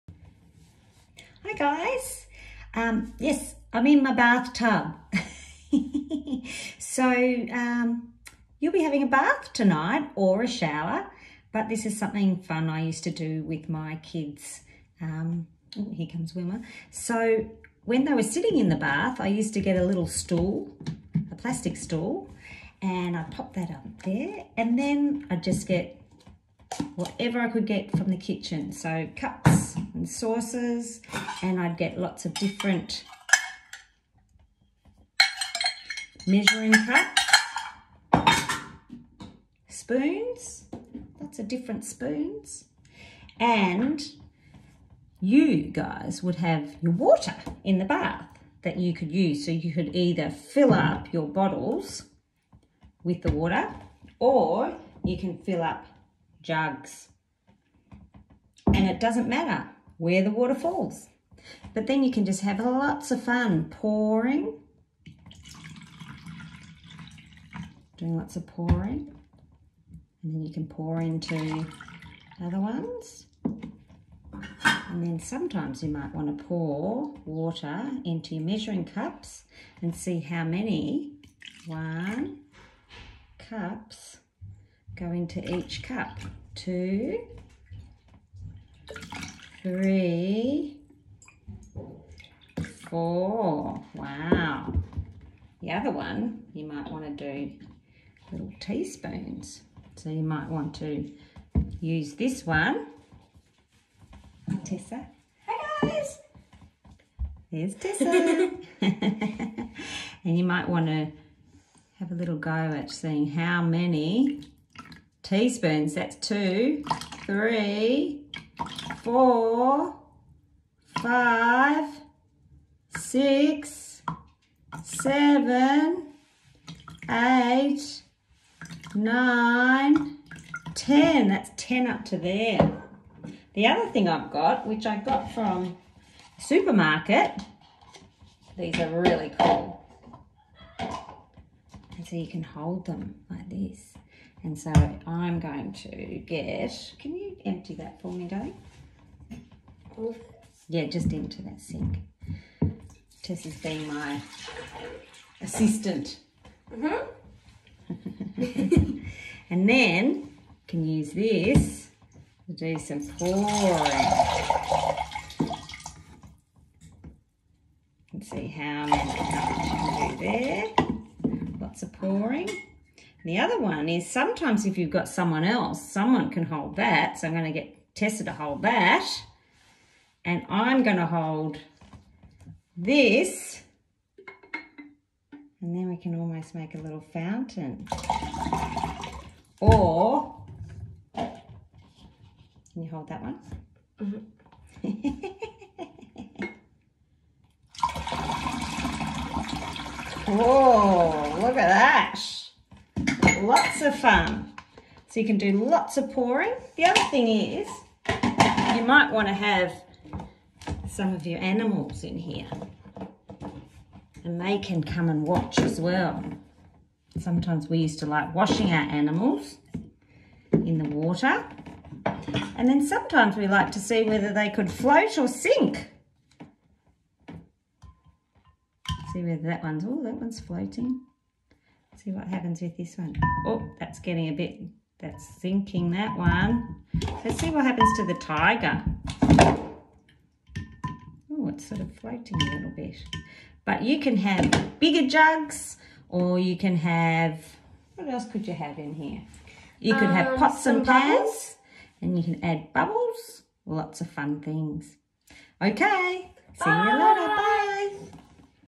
Play with Penny, bath time
Penny shows you how to have fun in the bath using items from around the kitchen. Children get to practice pouring water using various items. Children experiment with concepts such as volume, capacity as well as floating and sinking